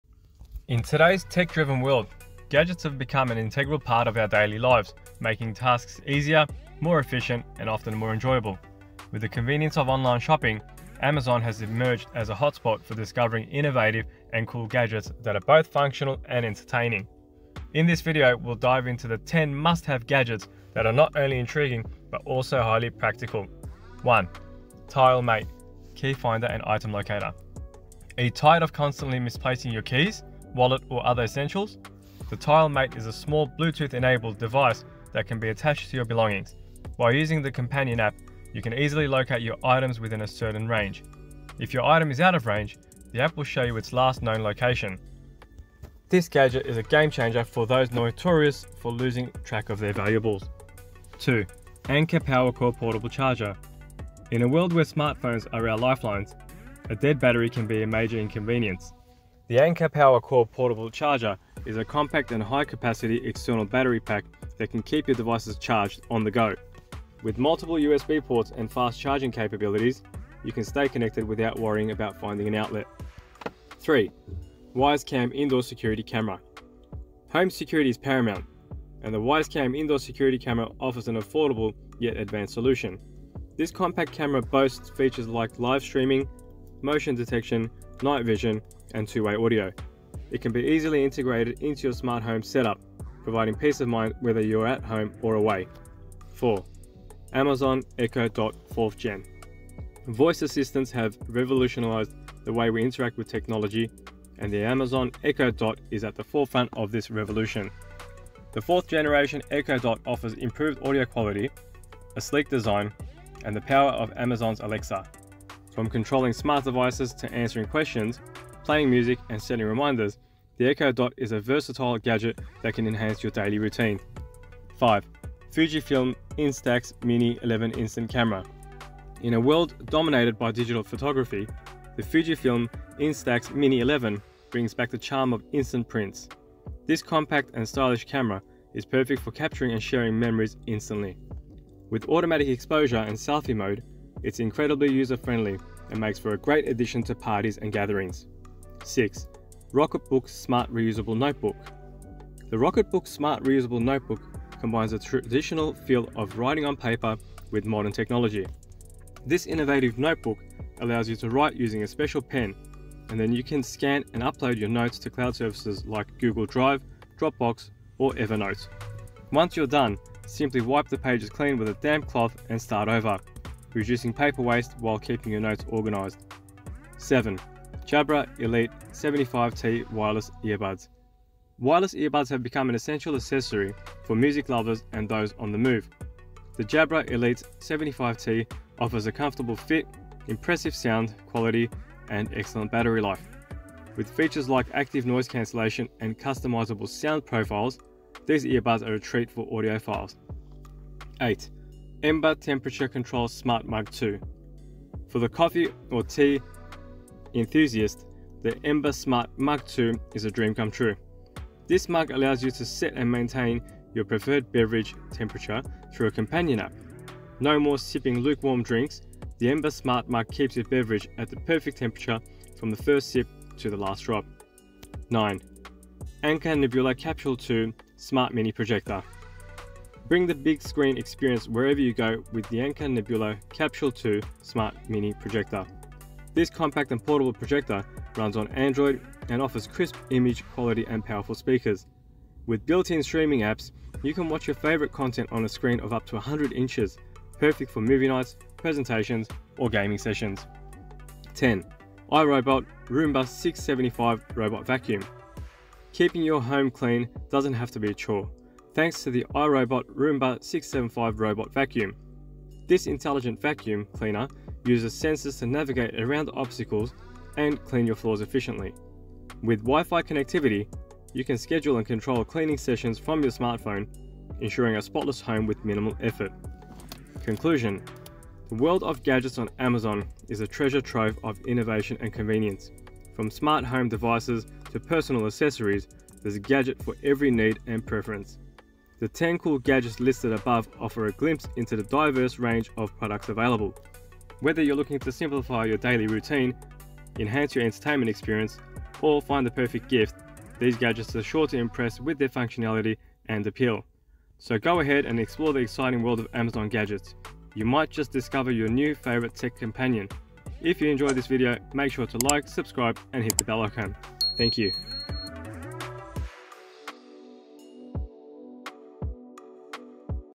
10 Must-Have Gadgets from Amazon That Will Upgrade Your Everyday Life
Whether you're a tech enthusiast or simply looking to make your daily routines more efficient, these gadgets are designed to enhance your lifestyle in remarkable ways.

 From cutting-edge smart home devices to portable and versatile accessories, we've curated a list that caters to every need.

Join us as we explore these ingenious creations and show you how they can transform the way you live, work, and play.